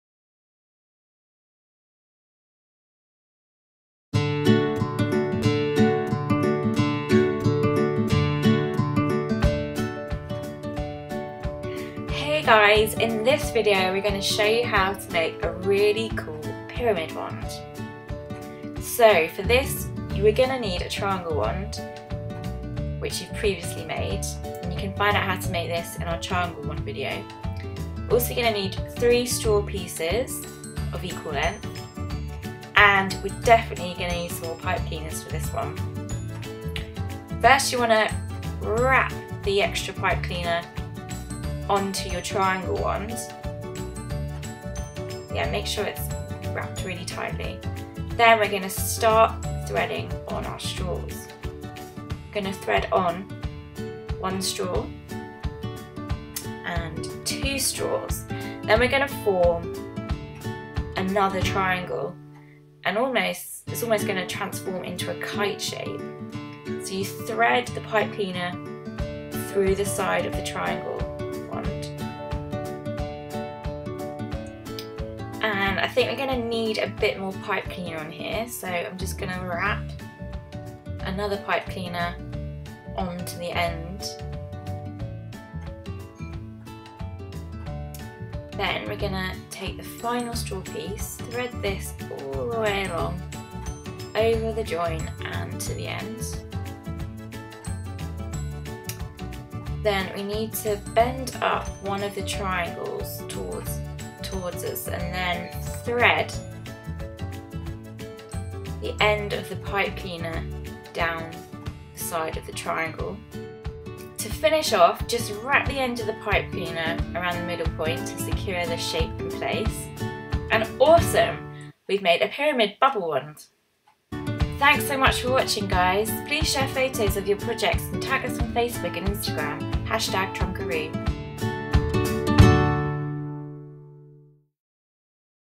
Trunkaroo Video Tutorial: 3D Pyramid Bubble Wand
Make a 3D pyramid shaped bubble wand